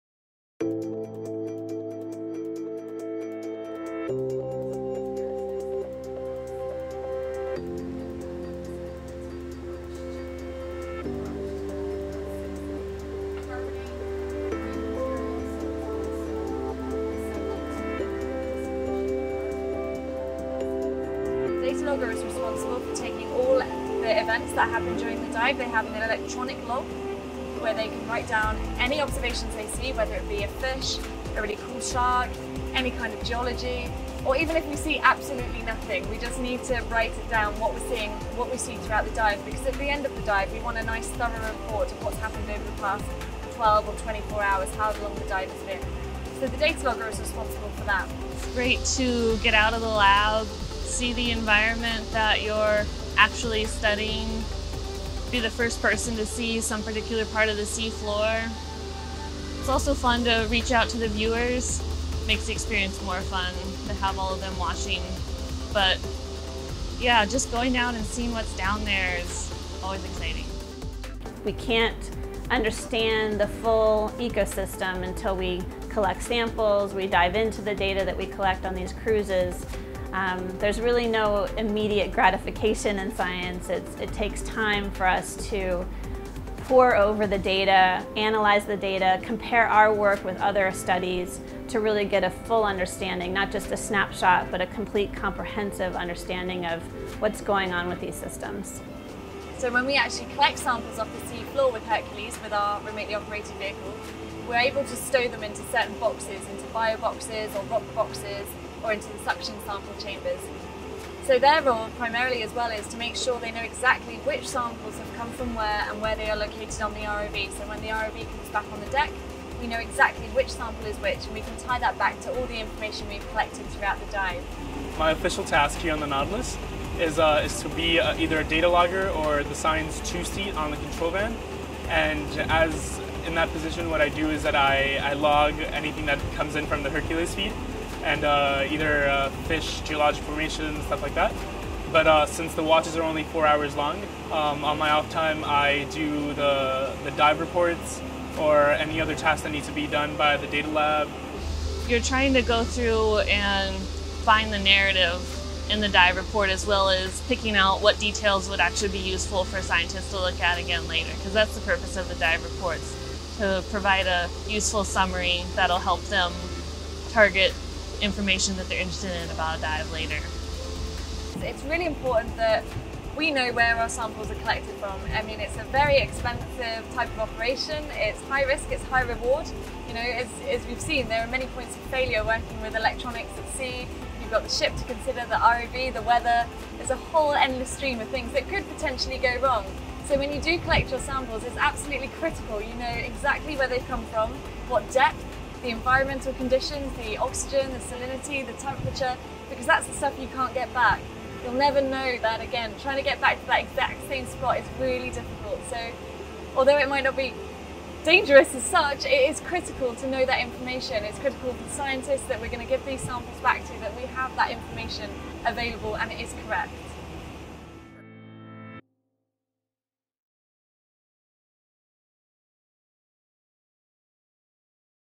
Data Logger Career Profile | Nautilus Live
It takes a lot of people to explore the ocean — at any particular moment E/V Nautilus can host up to 31 members of the science team and 17 ship’s crew, all contributing in various ways to the scientific efforts of the Corps of Exploration. No one is expendable — without pilots, cooks, navigators, mates, data loggers, engineers, and many more, the ship couldn’t function.

Learn more about what's it like to be a Data Logger, and about the many other STEM career roles on Nautilus